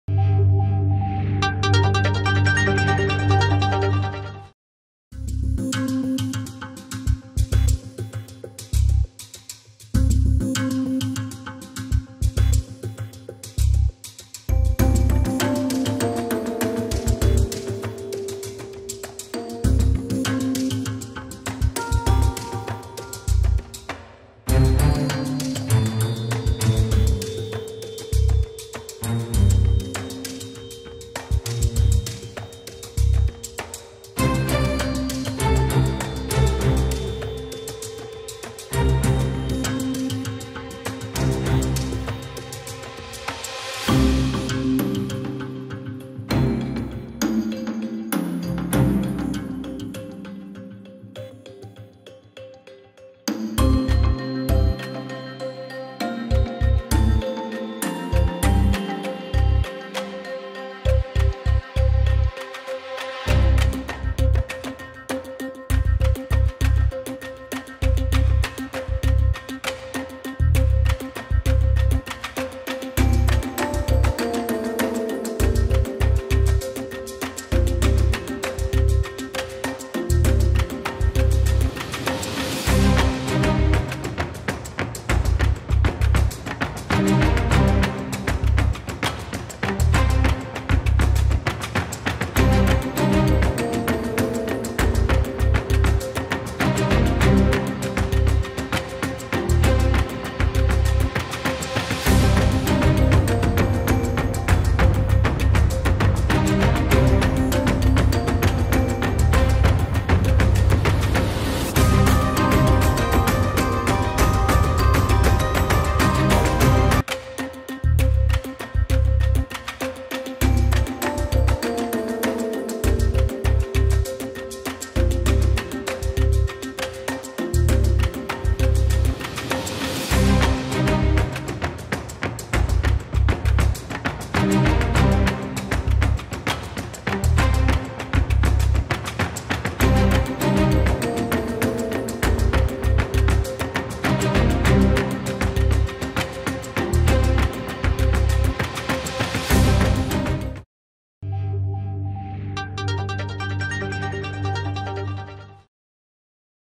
حملة أمنية بشوارع وأحياء كازا خلال فترة الليل
عاينت شوف تيفي الأحداث التي جرت فصولها ليلا بالبيضاء